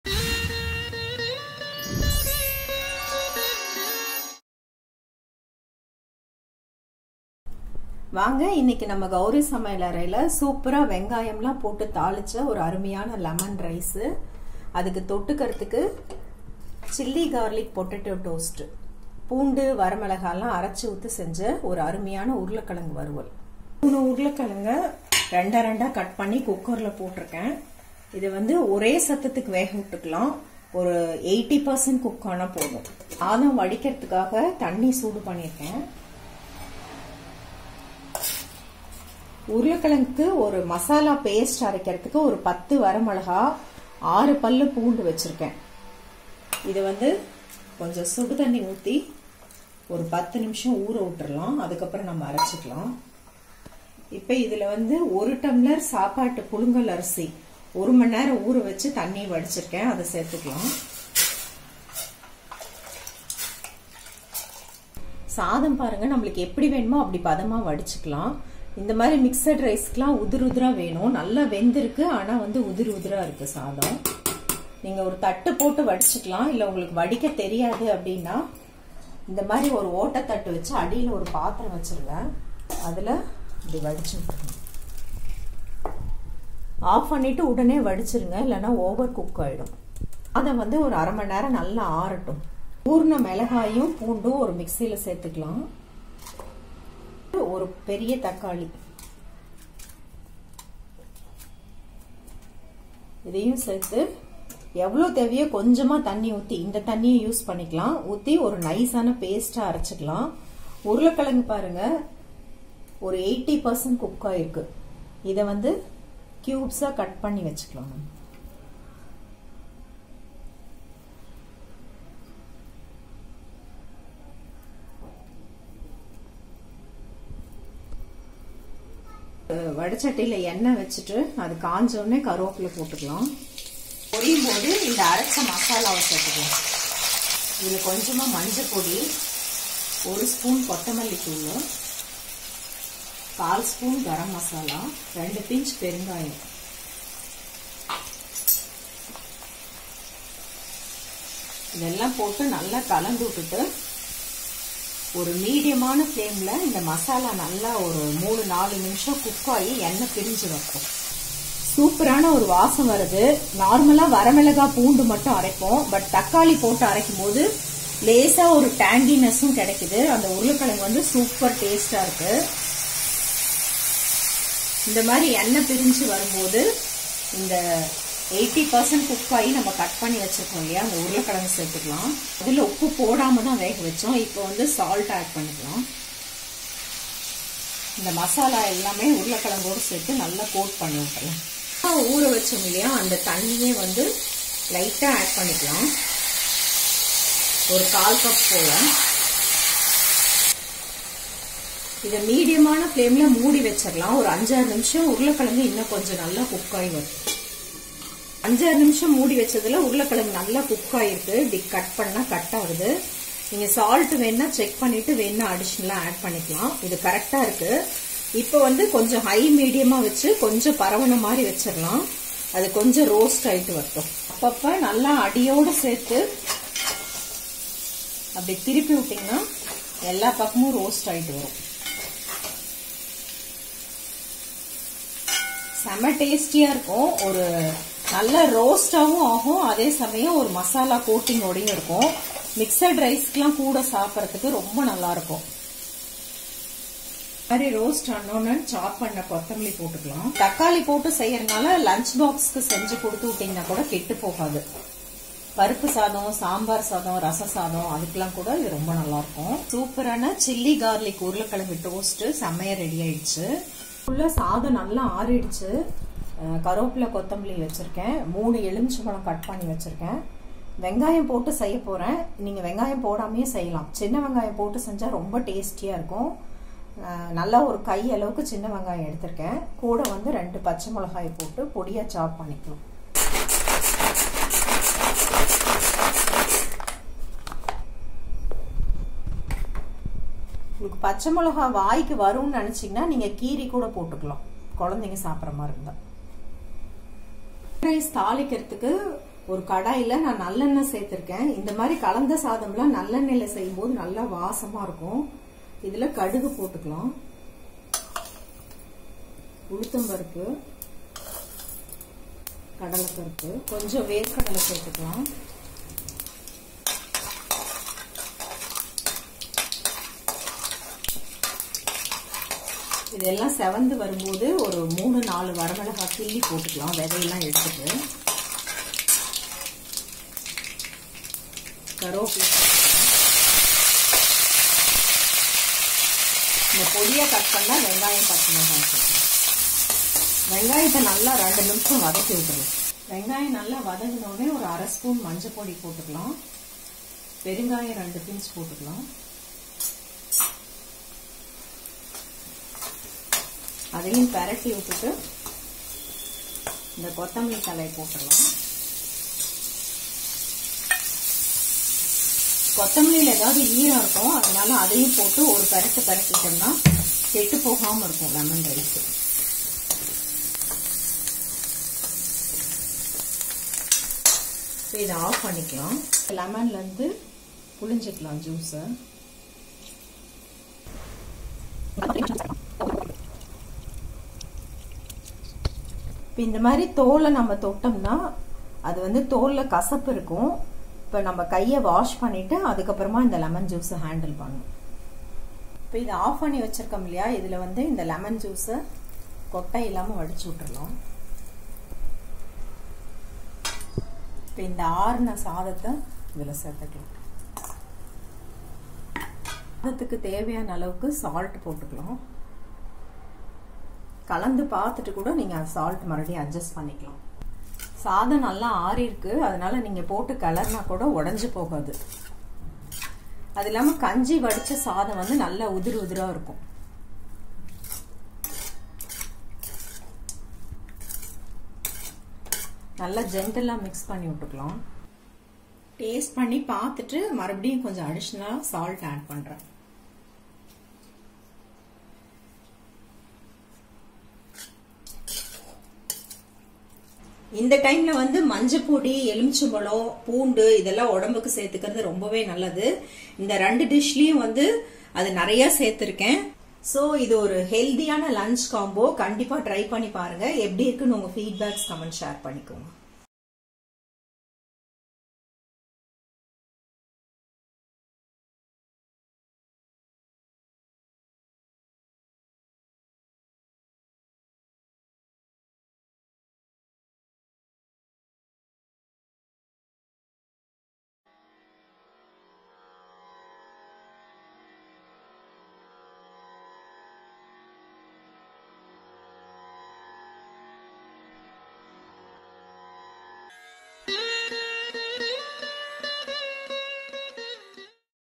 Lemon Rice Potato Roast Chilli Garlic Potato Roast Lunch Menu Gowri Samayalarai Tamil
Lemon Rice And Chilli Garlic Potato Roast

Nice Combo suits for lunch as well as lunch box.

Recipes

Lemon Rice

Rice - 1 cup (250 gm) Pulungal rice

Lemon 3 to 4

Roasted peanut - 2 tbs

Gingerly oil, mustard seeds, Ulundu Dhal, Gram Dhal, Hing, turmeric powder, salt, curry leaves, coriander leaves.

Wash and soak rice for 45 minutes. Boil 8 cups water, add rice, cook until 80 %. Drain the water, keep aside for 20 minutes until it cools down.
Chop onion, Green chilli finely. In oil fry mustard seeds, dhals, peanut, add red chilli, onion, Green chilli. Fry for few minutes. Add turmeric powder, Hing, curry leaves, coriander leaves, mix well, switch off.
Add cooked rice, salt, mix gently. Check for salt, add more, if needed. Delicious onion lemon rice is ready.

Chilli Garlic Potato Roast

Potato - 3 large

Garlic  6

Tomato 1

Oil, turmeric powder/4 tsp, coriander powder 1 tsp, garam Masala powder1/4 tsp, Hing, salt, curry leaves, coriander leaves.

Soak red chilli, Garlic in hot water for 10 minutes. Stain the water, add tomato, grind to a smooth paste.
Cook potato for one whistle, peel the skin, cut into cubes.
In oil fry curry leaves, add grinded masala, all powders, cook for few minutes, until oil separates. Add potato, salt, mix well, add 1/2 cup water, cook for 5 minutes, in low flame. It is cooked and become dry, put in high flame and spread it, roast it both sides, by turning often. Add coriander leaves, mix, switch off. Delicious Potato Roast is ready.

With love
Gowri

To see more recipes in Gowri Samayalarai